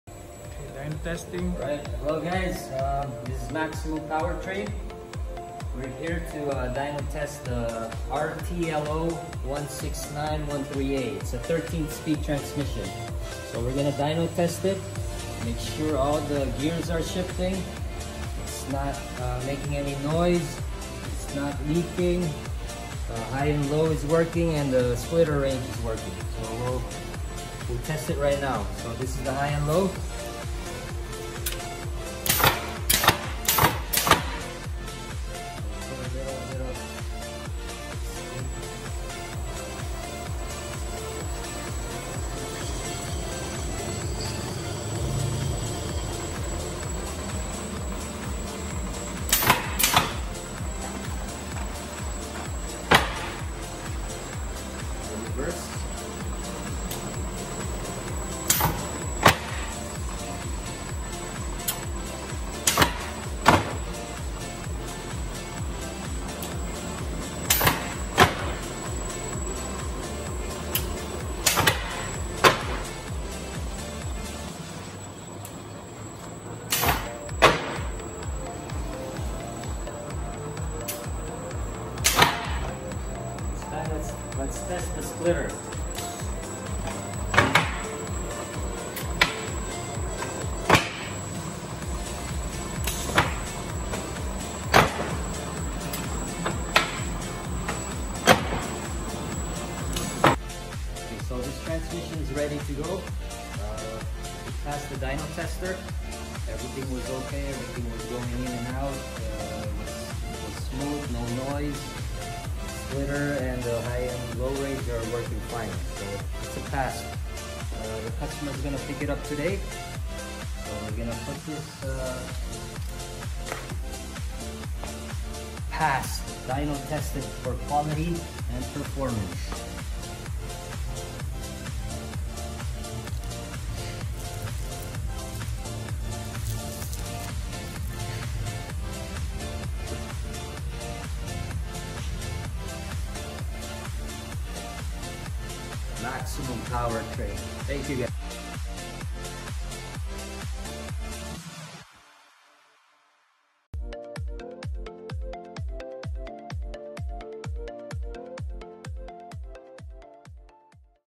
13 speed transmission Dynotester
How to shift and dynotest Eaton fuller 13 speed transmission RTLO16913A